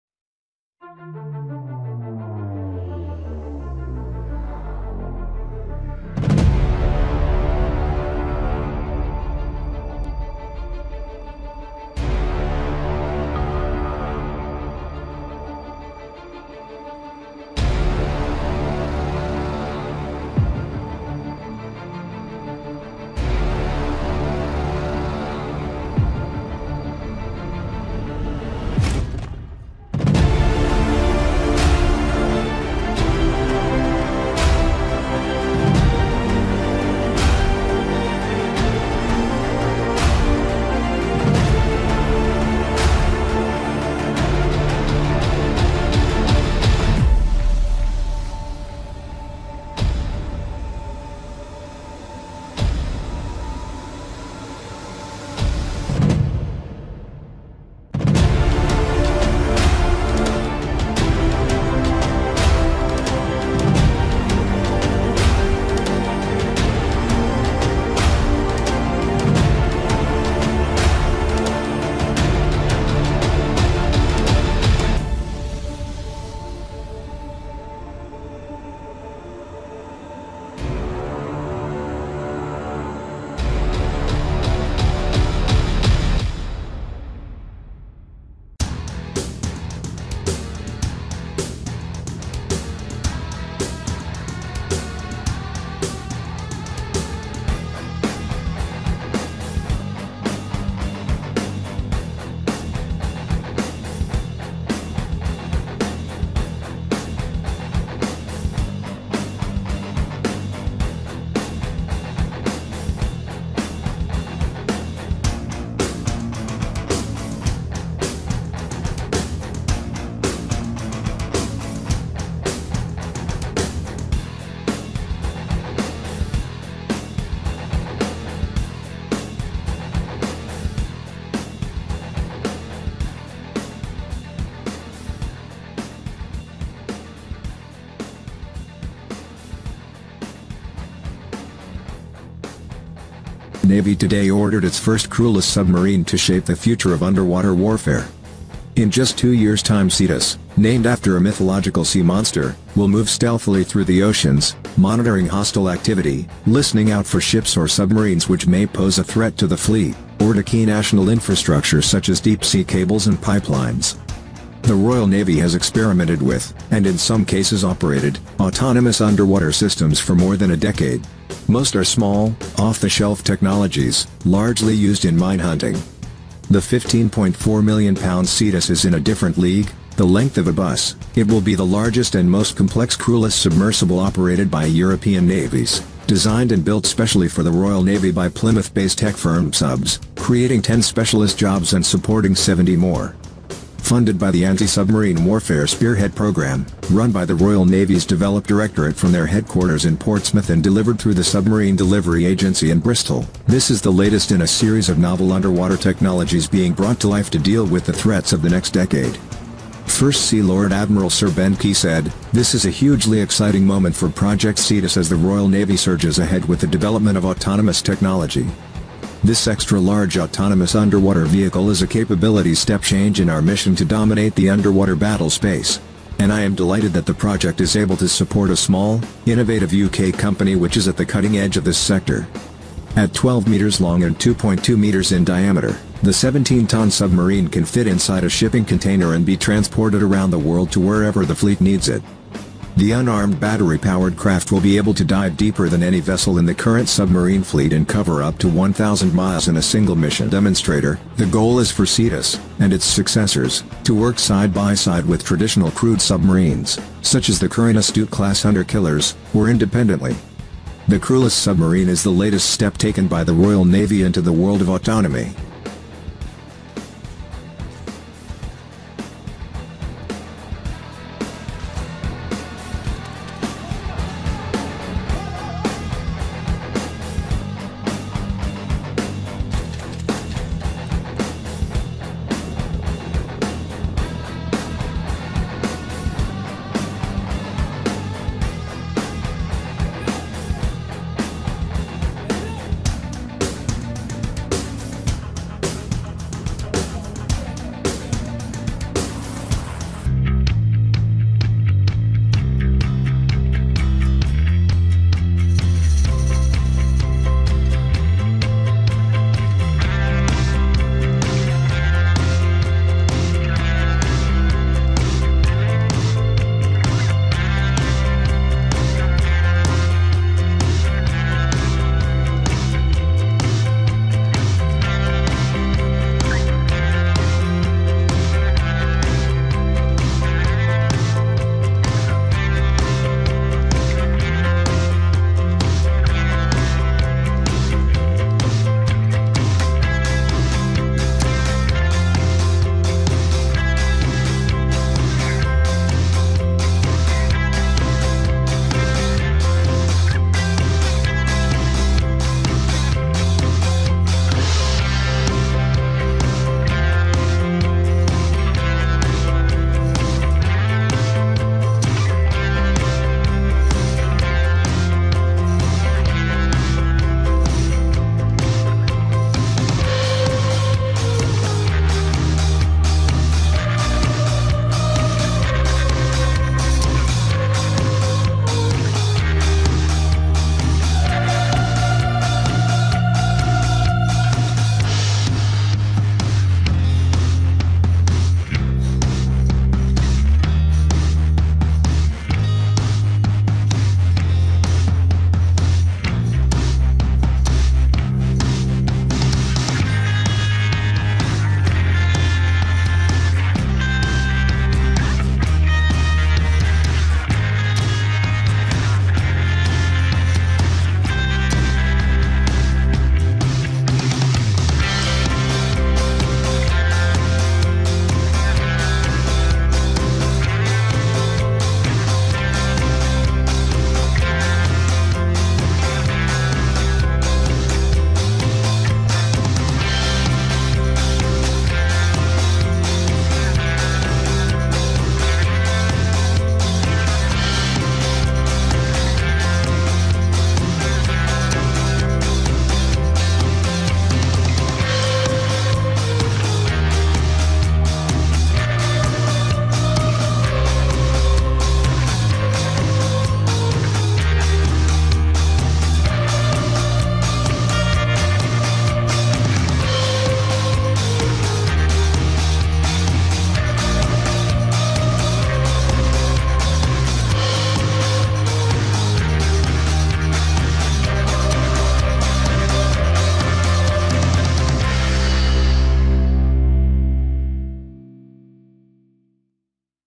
UK Navy to take drone teaming operations underwater with new submarine
A £15.4 million contract for a cutting-edge crewless submarine has been awarded to Plymouth firm MSubs by the Royal Navy. The vessel will be delivered to the Navy in two years’ time and will further advance the UK’s ability to protect our critical national infrastructure and monitor sub-sea activity.

Project Cetus – named after a mythological sea monster – enhances the Royal Navy’s experimentation with autonomous underwater systems. It is the first step in developing an operational autonomous submarine that will work side-by-side with crewed submarines – including the Astute-class hunter-killers and their successors – or independently.